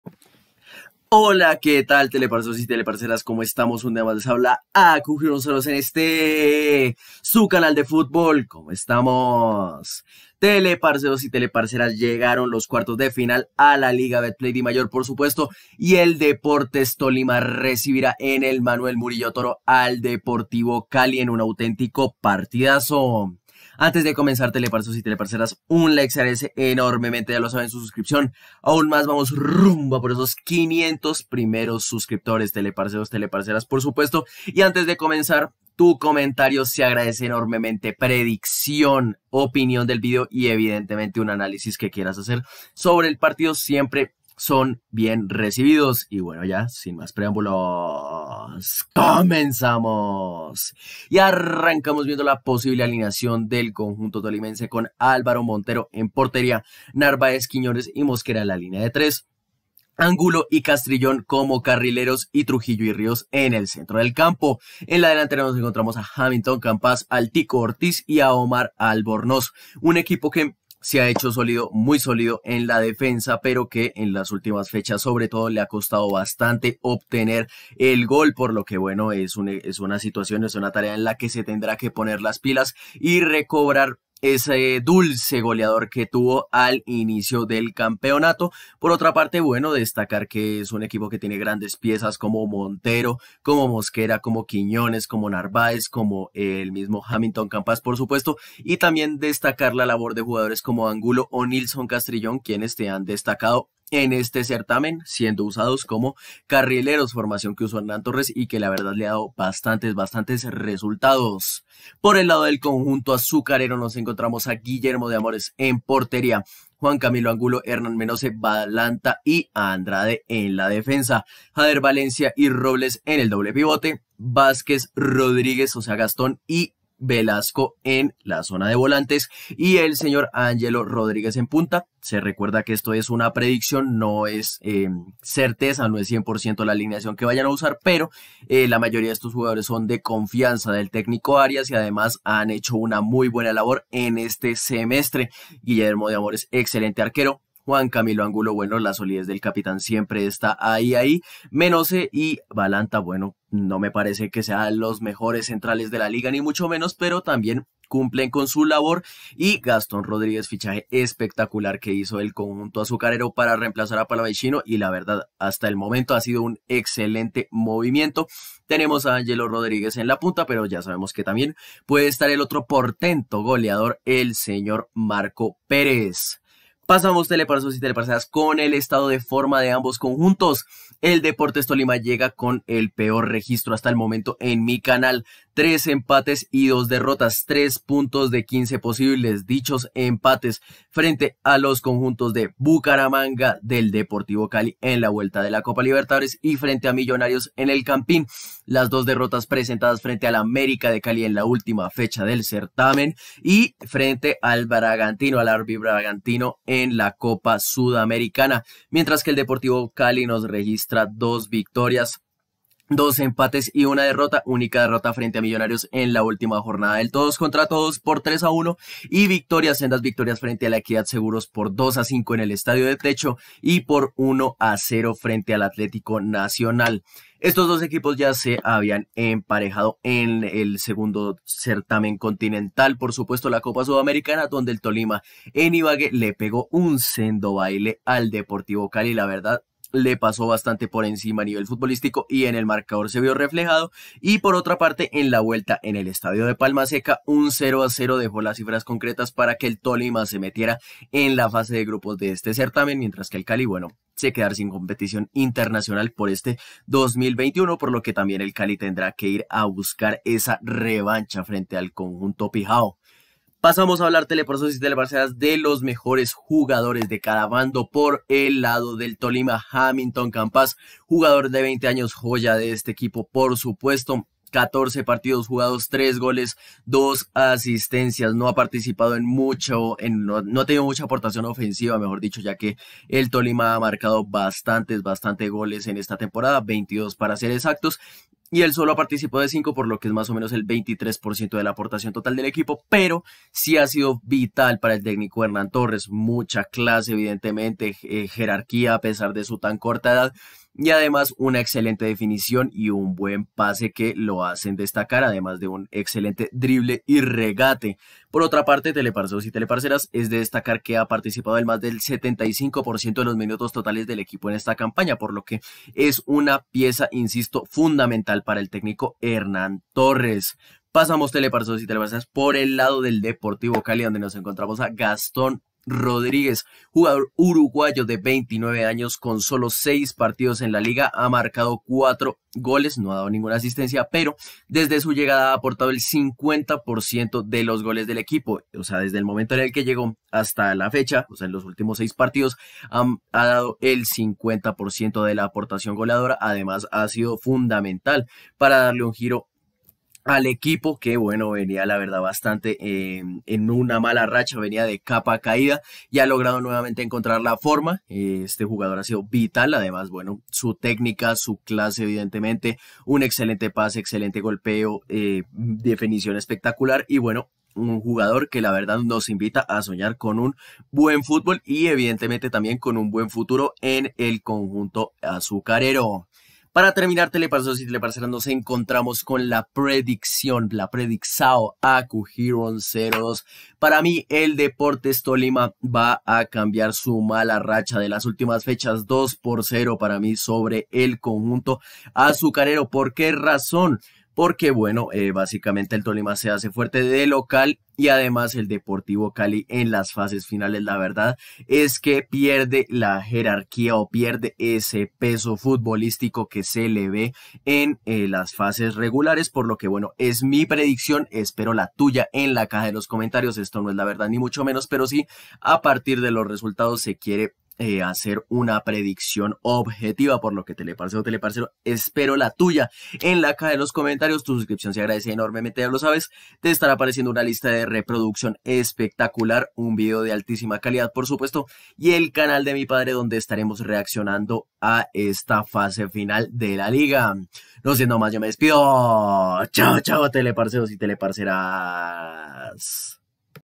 🐷DEPORTES TOLIMA vs DEPORTIVO CALI🟢| Análisis y Predicción | Liga Betplay 2021 Cuartos de Final IDA
El Tolima y el Cali se enfrentan en los cuartos de final de la LigaBetplay 2021 con el objetivo de clasificarse a semifinales y a la postre encarar la gran final del torneo y si se puede soñar, obtener la estrella de mitad de año.
El Tolima viene a este partido luego de caer en Brasil 2-1 frente al RB Bragantino, mientras que el Deportivo Cali llega tras caer por 3-1 frente a Millonarios.
El ganador de esta serie se enfrentará al ganador de Atletico Nacional vs Equidad.